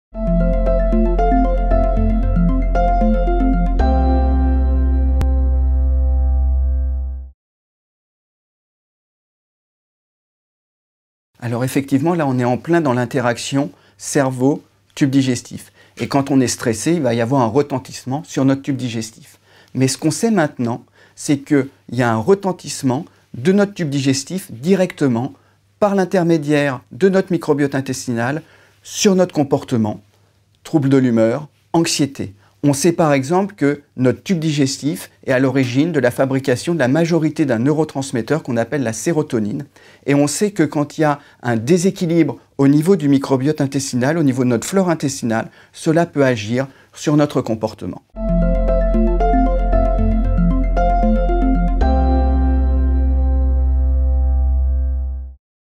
Les liens entre le cerveau et le tube digestif
Saviez-vous que la composition du microbiote intestinal influe sur notre comportement ? Les explications du Pr Gabriel Perlemuter, chef du service d’hépato-gastro-entérologie à l’hôpital Antoine Béclère (Clamart).